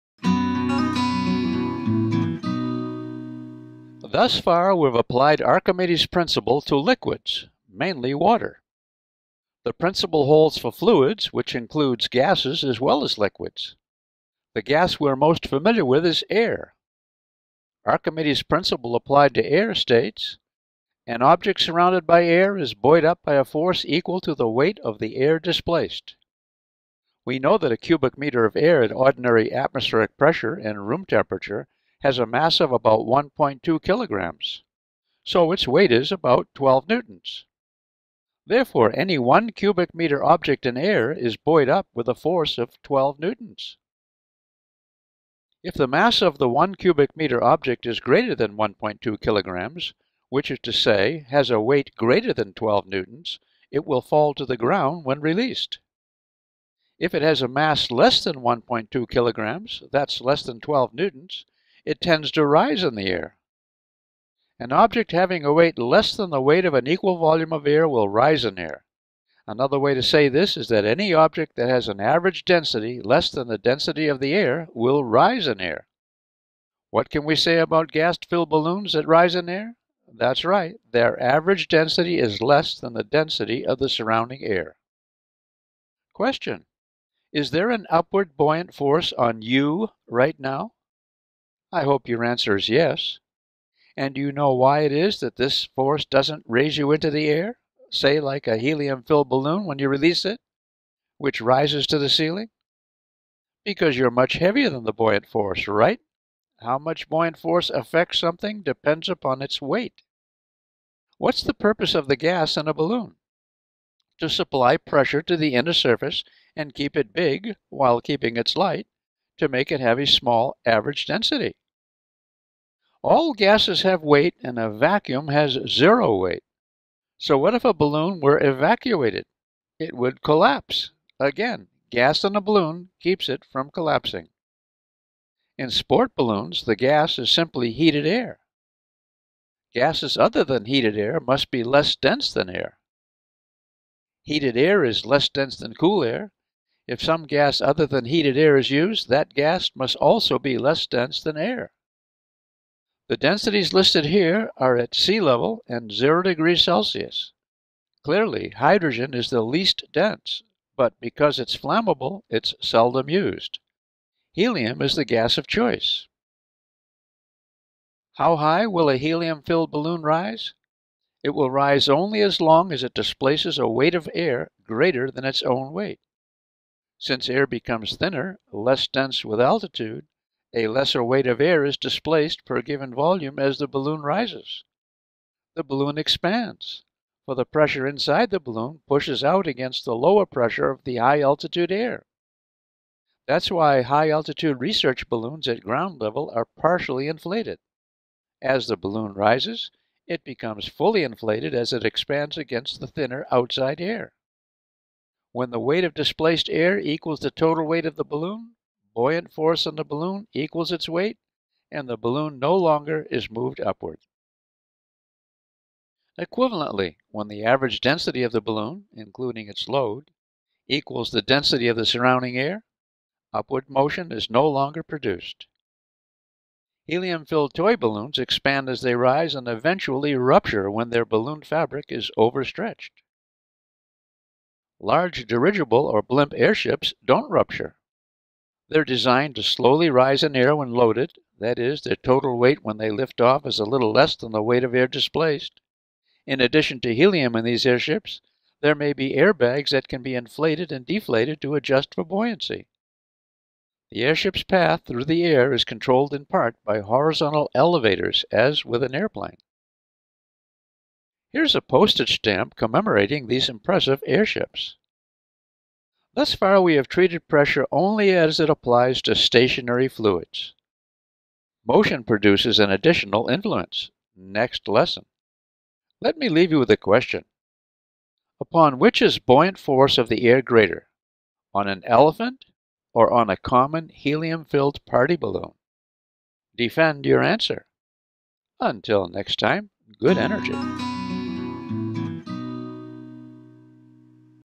Hewitt-Drew-it! PHYSICS 67. Buoyancy of Balloons
Problem solving with the buoyancy of ascending balloons is investigated.